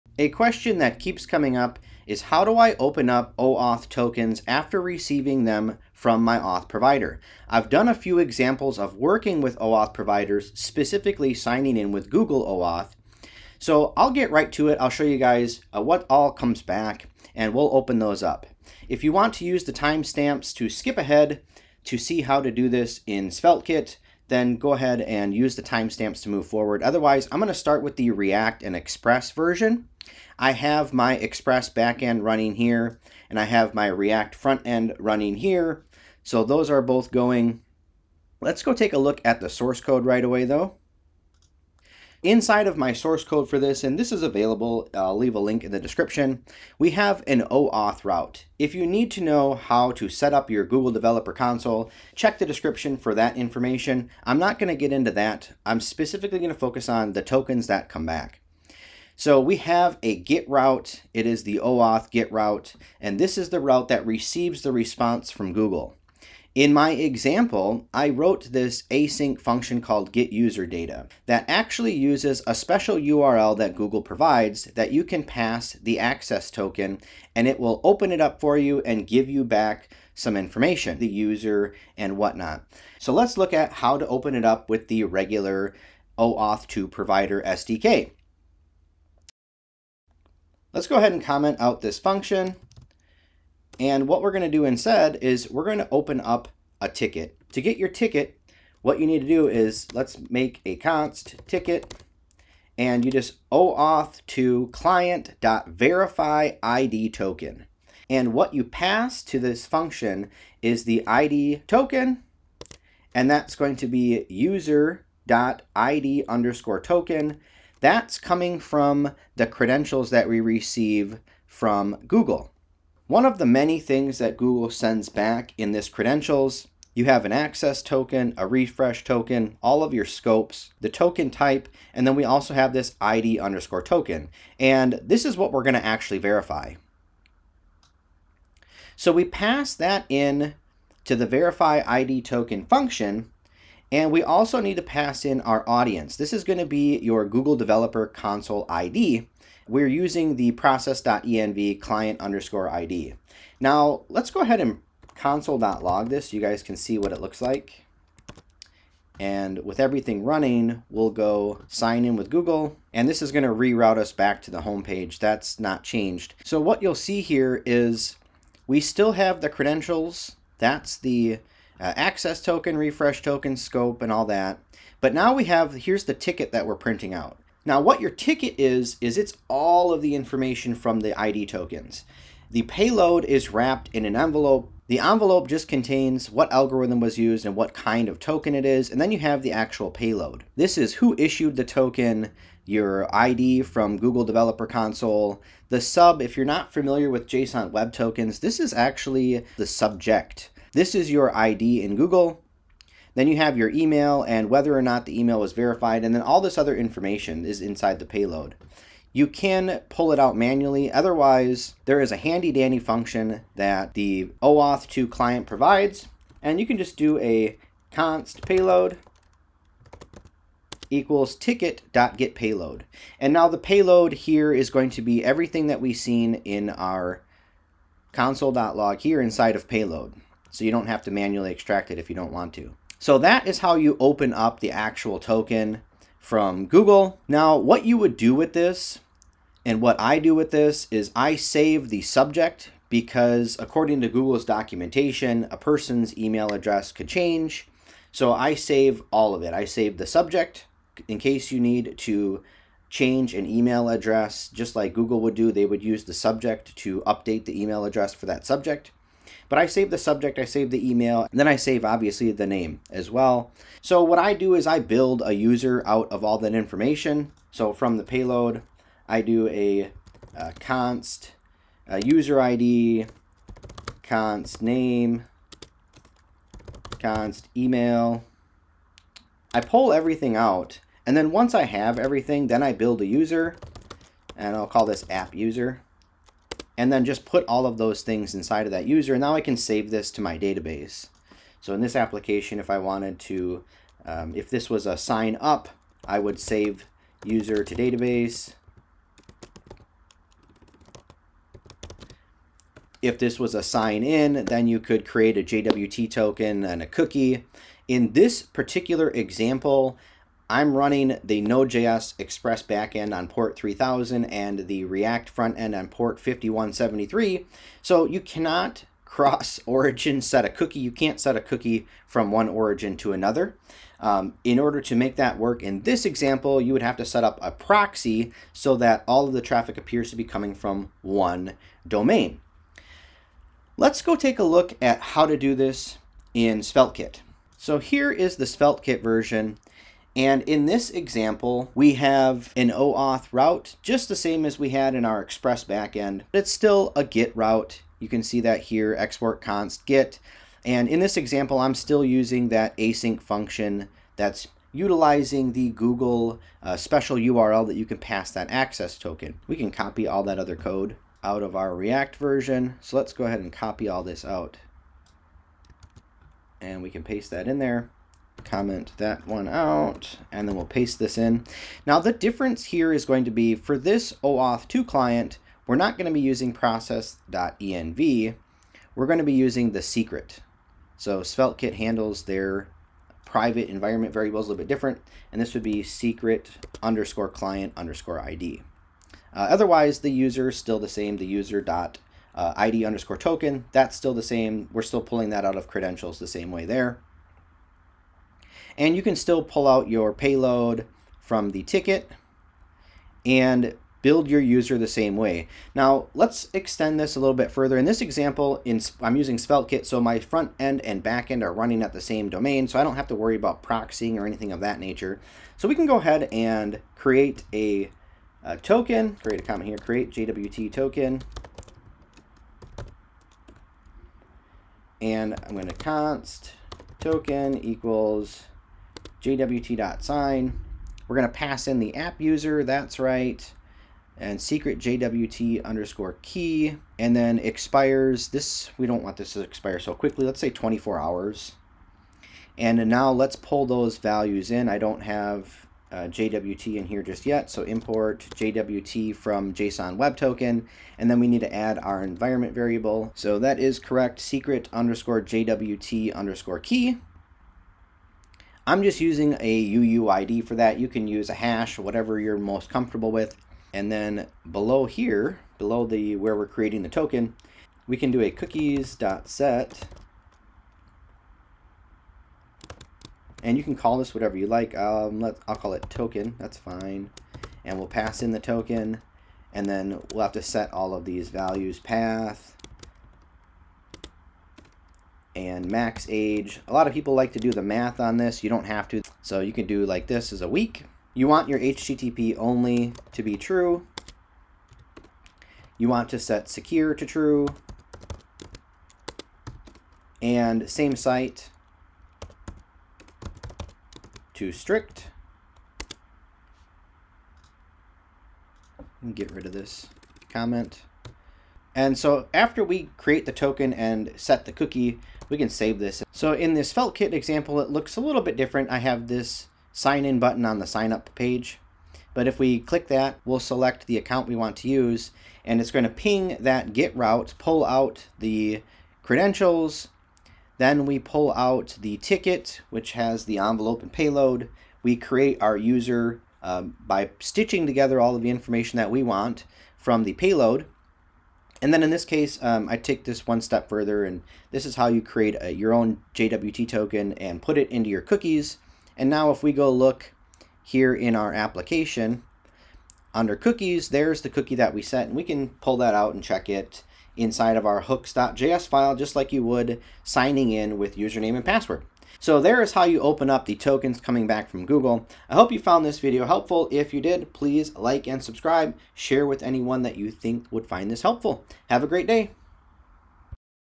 OAuth2 JWT Tokens: How to Open Tokens / What is inside
"🚀 Dive into the world of Google OAuth2 Tokens with my latest video! Join me as I break down opening OAuth2, explore JSON Web Token format. 🔐 Security is cool, and we've got the best practices to prove it! 🤖 Embrace the future of web development with OAuth2. 🌟 Ready to become a coding maestro? Hit play now and unlock the tech wizard within! 🚀✨ TechTalk OAuth2Magic CodingJourney DeveloperLife GoogleAPI"

05:30 - SvelteKit